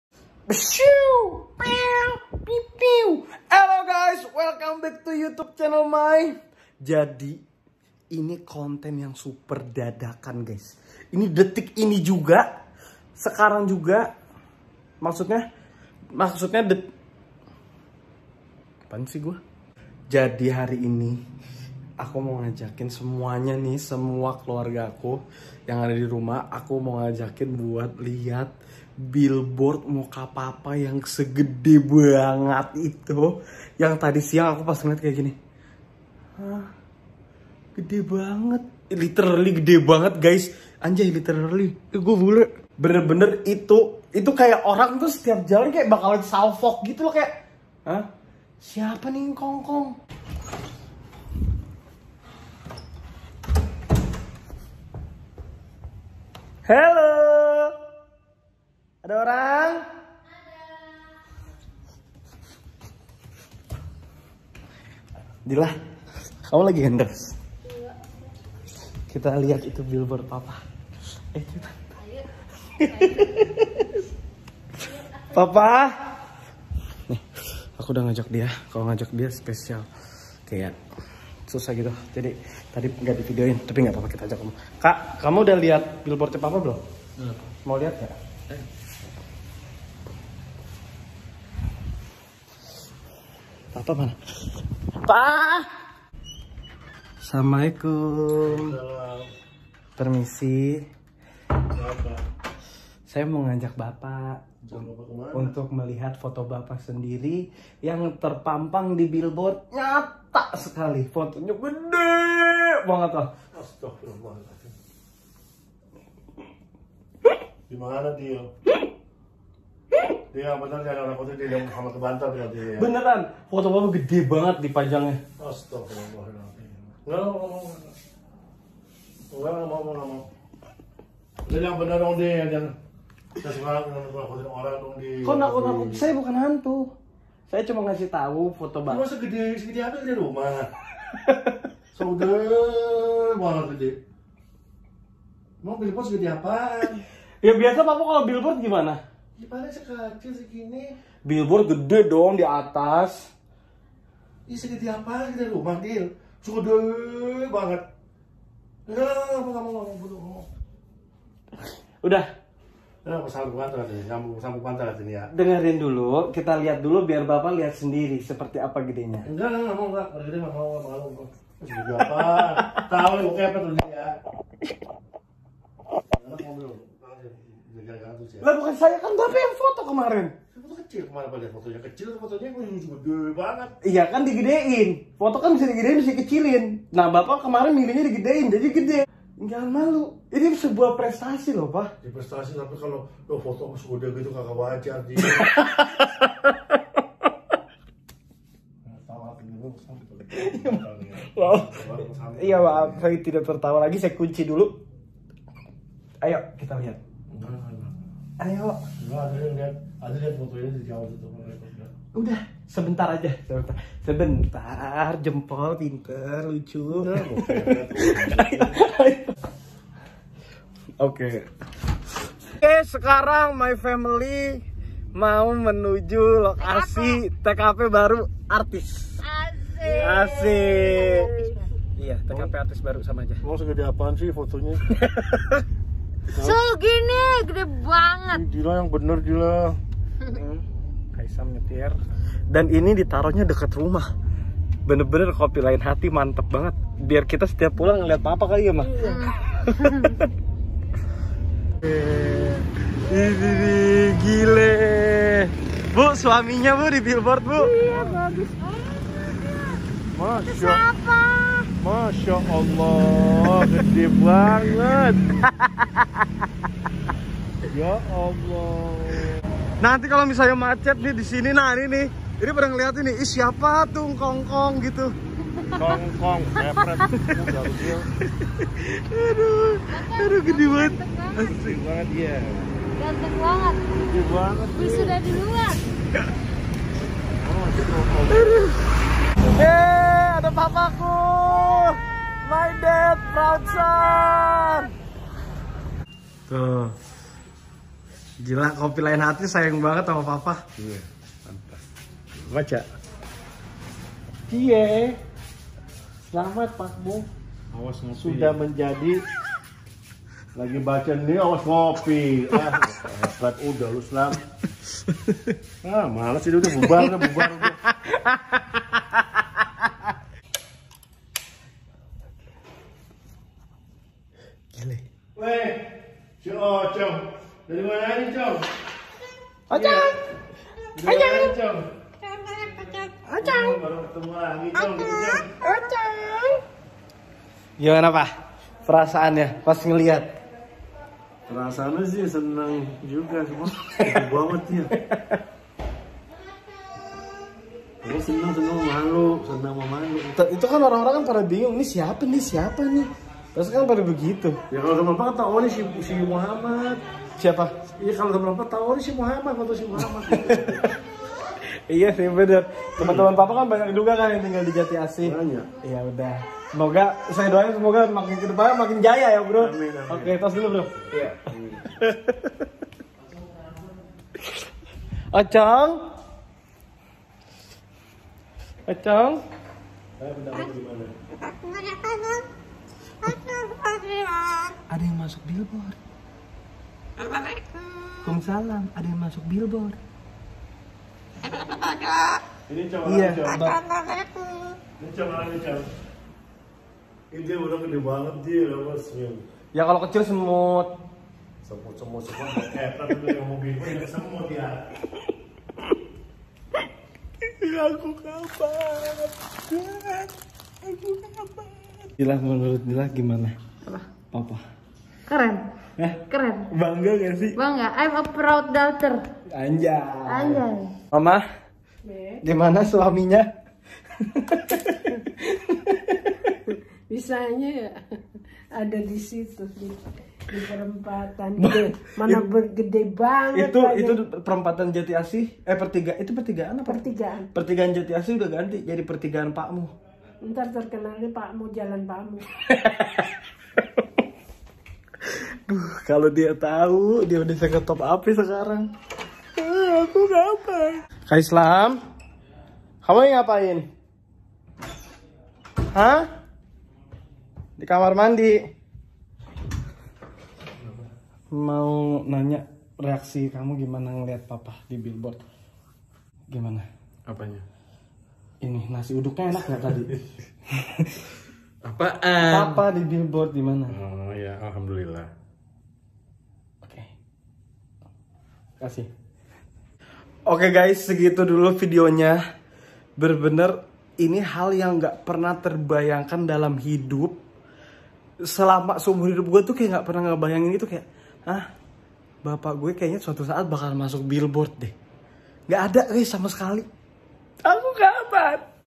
FADIL JAIDI ‼ PAK MUH MASUK BILBOARD, WKWKWKWK
Pak Muh Akhirnya Direkrut Ķopi LainHati Sebagai Brand Ambassador..
Pak Muh sepertinya Lebih Tenar dari Fadil Jaidi,Gimana Keseruannya?
Cekidot.
Terimakasih udah nonton☺